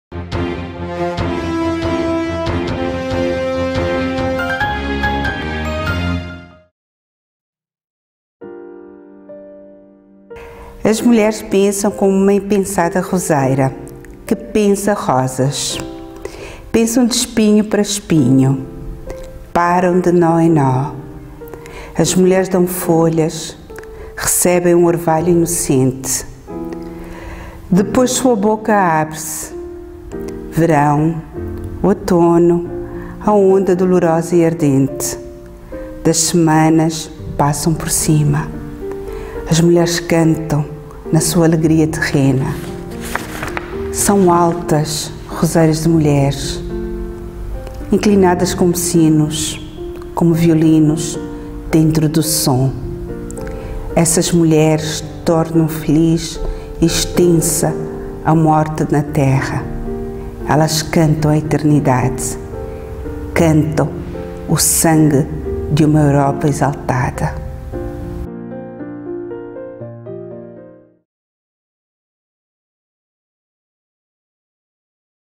Ep371 Comemorações do Dia Internacional da Mulher MARIA EVA ADAMS
No âmbito das comemorações do Dia internacional da Mulher, a Junta de Freguesia de Carnide e a CarnideTV, desafiaram várias mulheres de Carnide a declamarem um poema inspirador sobre as mulheres.